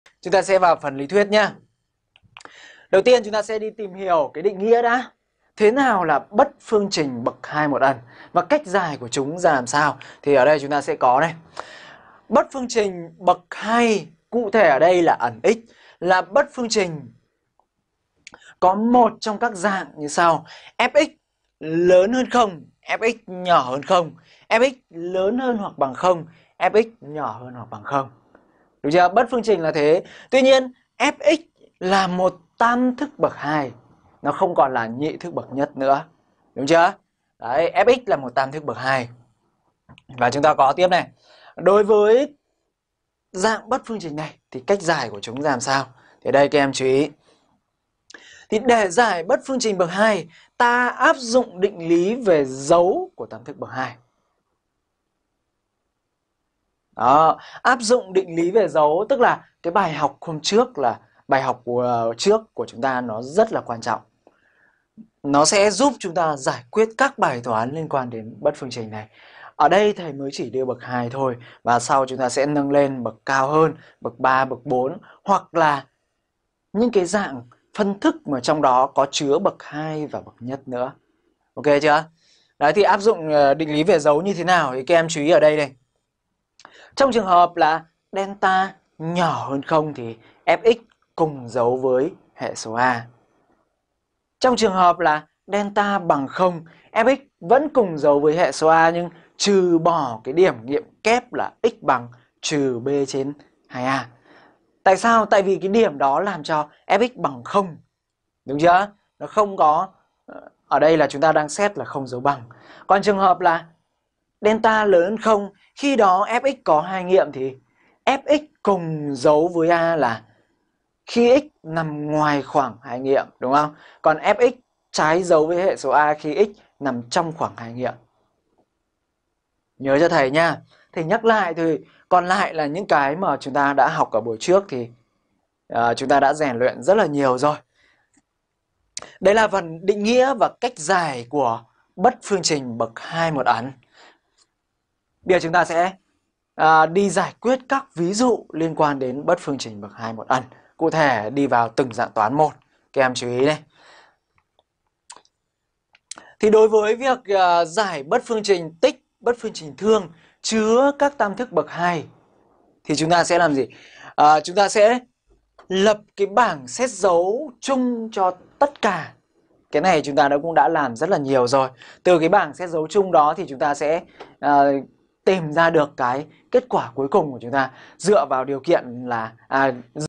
Bất phương trình bậc hai - Bất đẳng thức - Bất phương trình - Đại số 10 LT
Bất phương trình bậc hai

- Câu châm ngôn: trên bước đường thành công ko có dấu chân của kẻ lười biếng.
- Quan điểm: Dạy toán là dạy cách tư duy,phân tích
- Số khoá học trên trang: 03